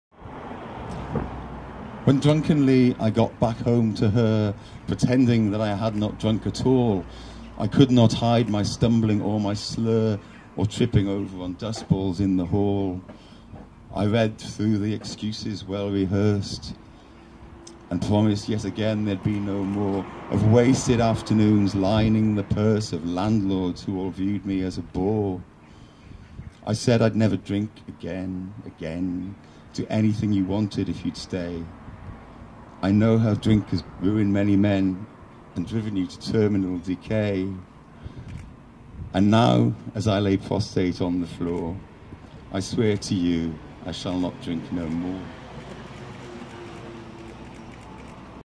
Anthony Jones på Pilsnerpoesi 15 06 27
Pilsnerpåesi på Ban Thai Tranås 15 06 27 med speciell gäster Christer Boberg och Anthony Jones.